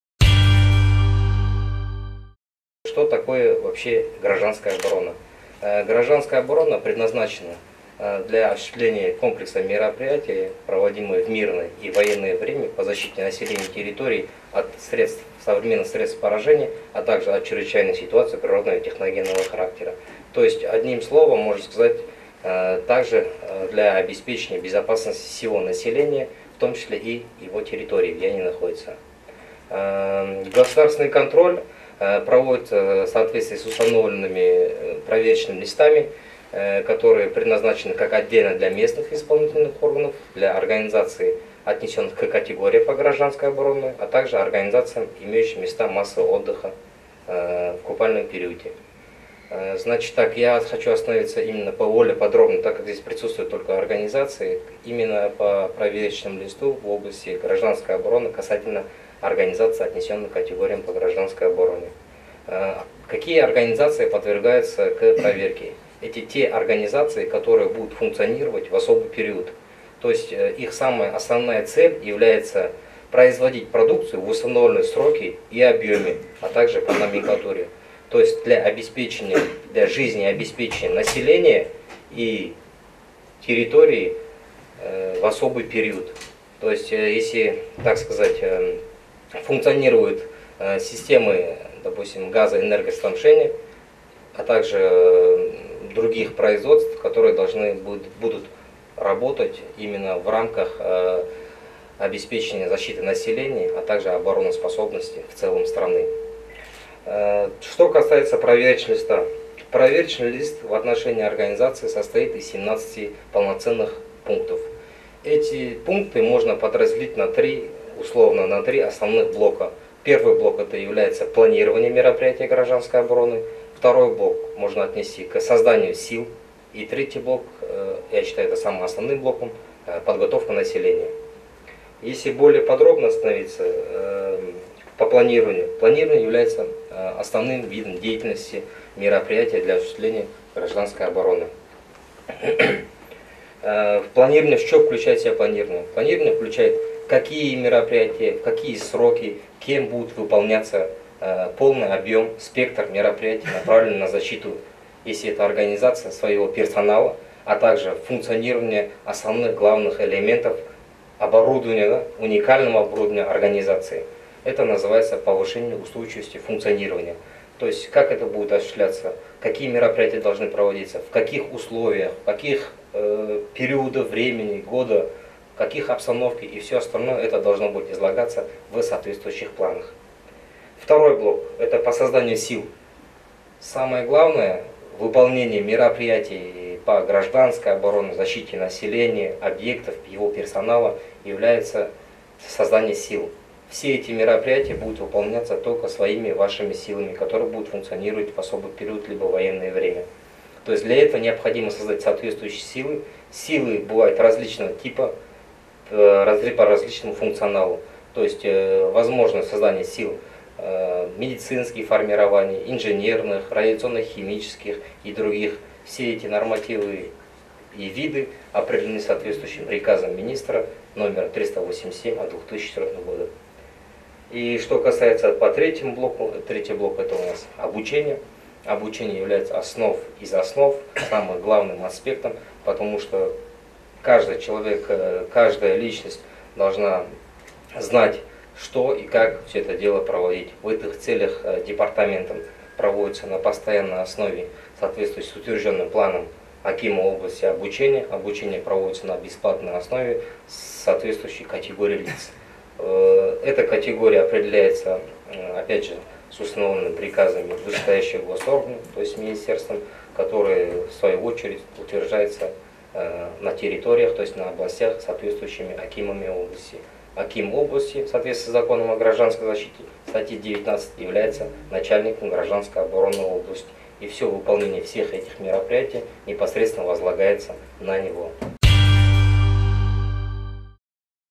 Вопросы противопожарной защищенности объектов озвучены на встрече с предпринимателями в ДЧС
Видео: Айдара Кабдол